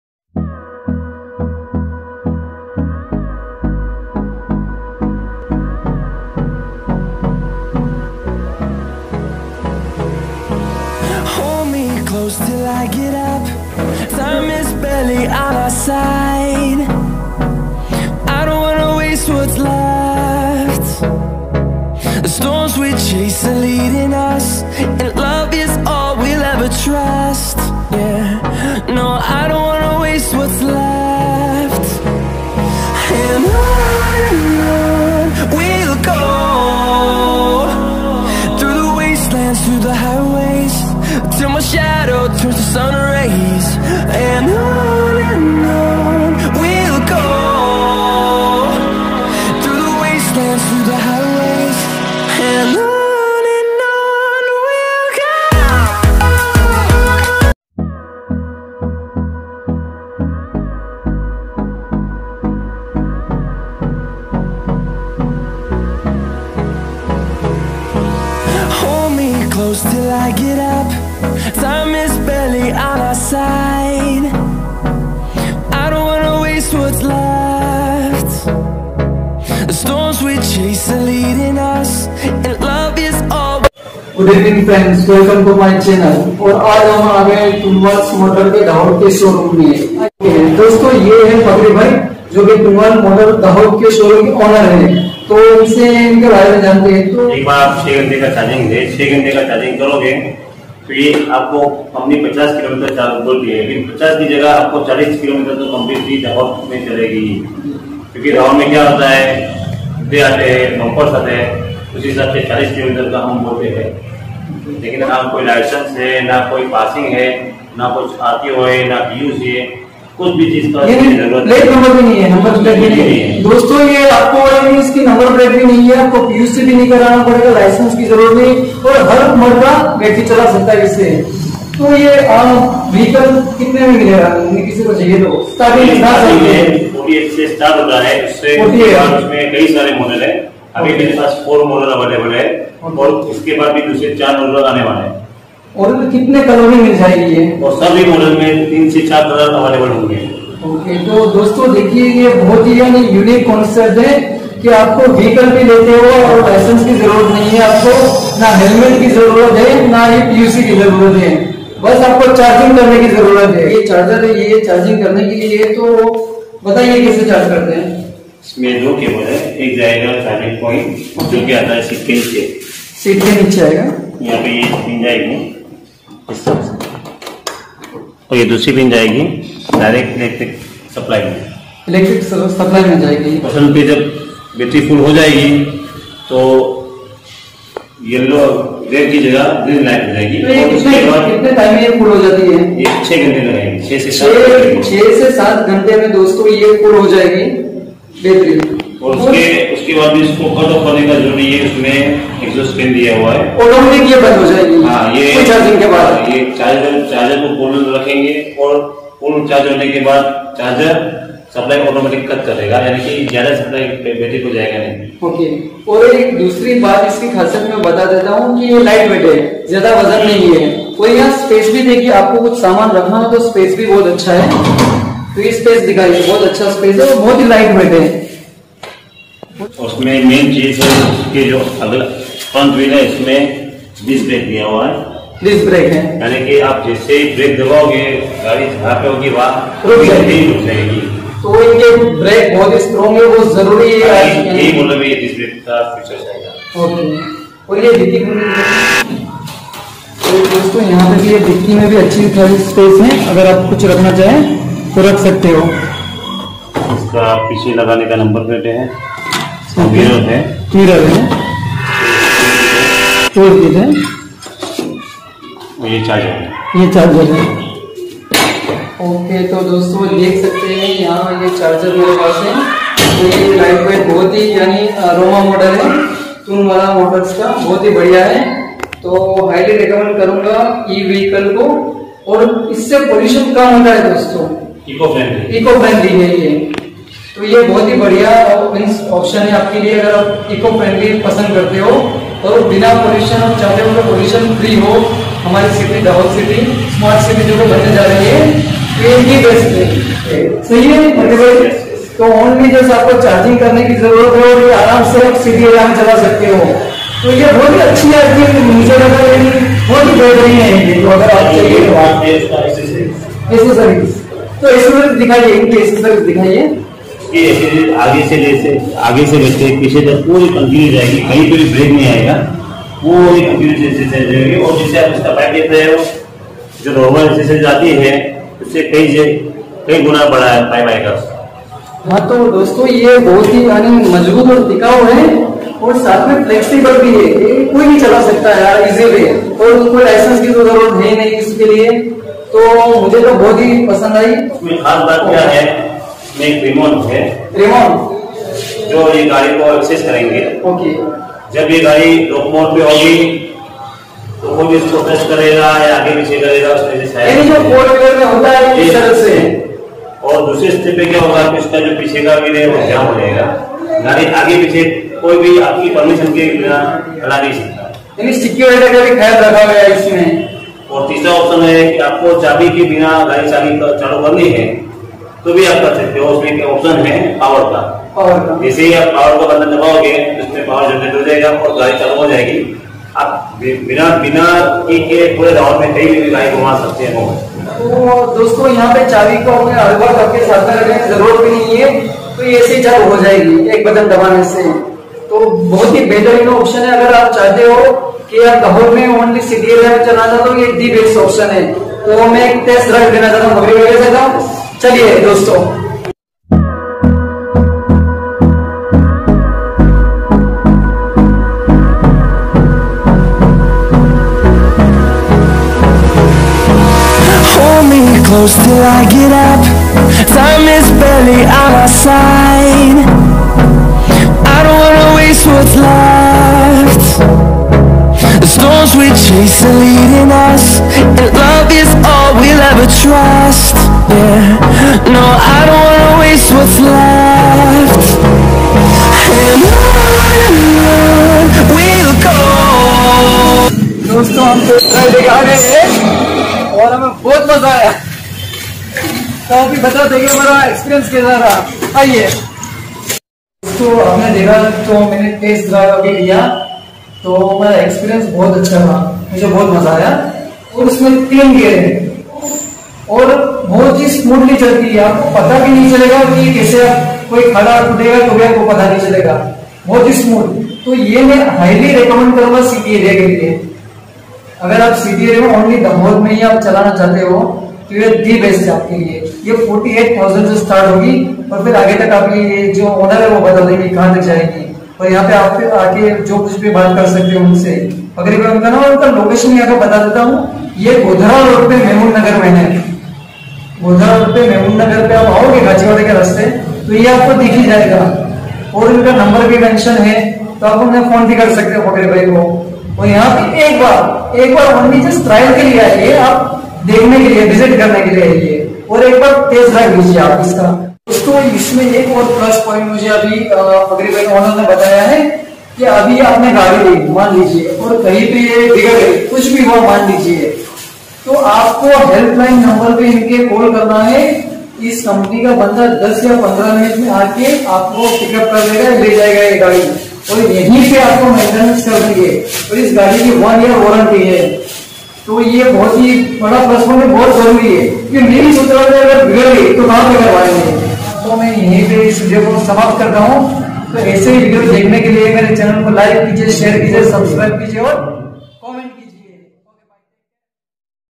TUNWAL E - MOTORS | RAJ E - VEHICLES | DAHOD
there's electric vehicles available in dahod, godhra road,  memoon Nagar
it's very good for in dahod city